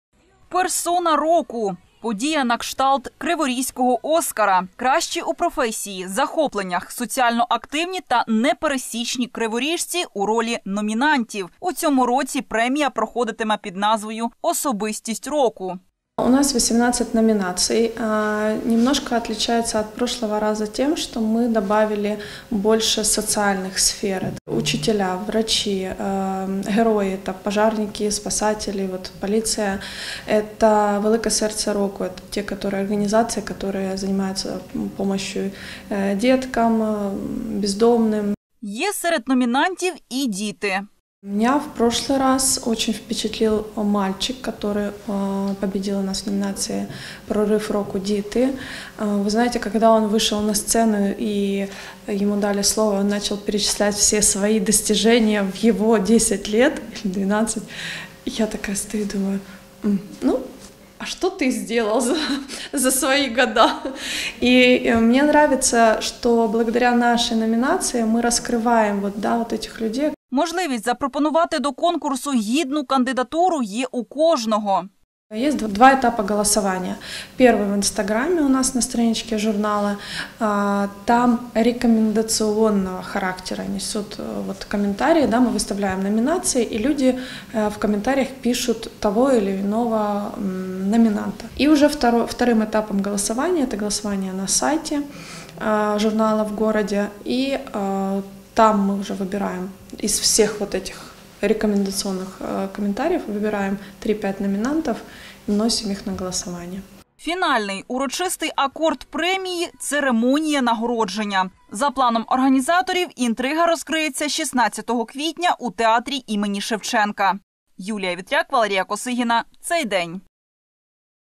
Особистість року: у Кривому Розі готуються до урочистого вручення премії
Менше як за місяць у Кривому Розі відбудеться урочисте вручення премії «Особистість року 2021». Цього разу переважають номінації соціального спрямування. Докладніше - Юлія Вєтряк.